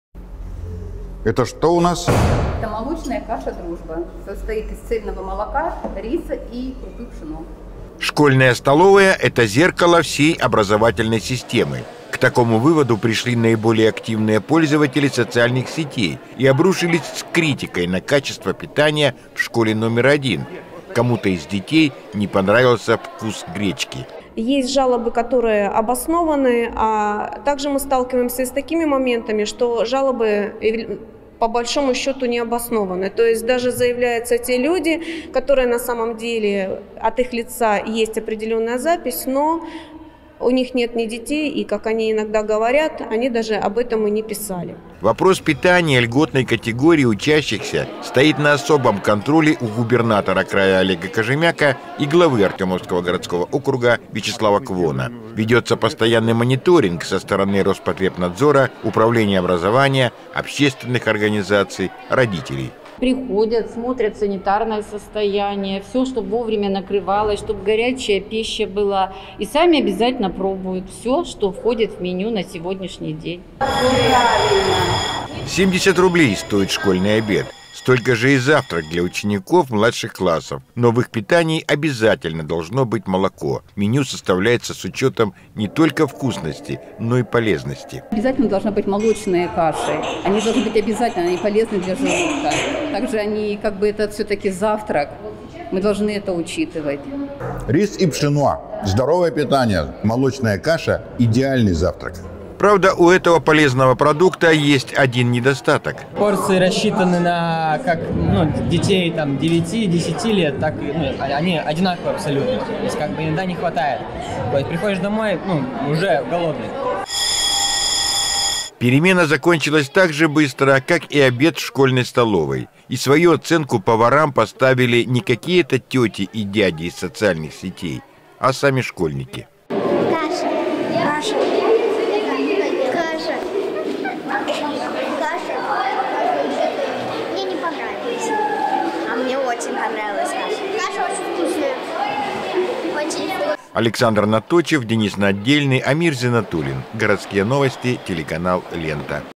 Школьная столовая - зеркало образовательной системы
Вопрос питания льготной категории, стоит на особом контроле у губернатора Приморского края Олега Кожемяко и главы Артёмовского городского округа, Вячеслава Квона. Ведётся постоянный мониторинг со стороны Роспотребнадзора, общественных организаций, родителей. Для проверки неподтверждённых данных, в школу отправились сотрудники управления образования администрации города. Вместе с ними съёмочная группа телеканала «Лента»

Одноклассниках -  http:/ok.ru/ctcartem